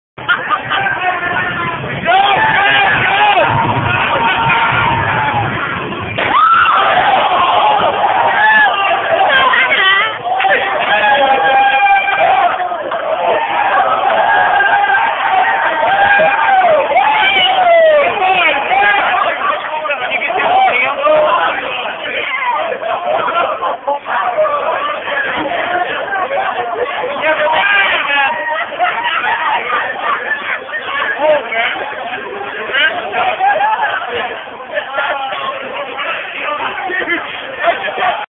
Mayfest Fall - Face Meets Pavement
A friendly town race ends with a fail!!!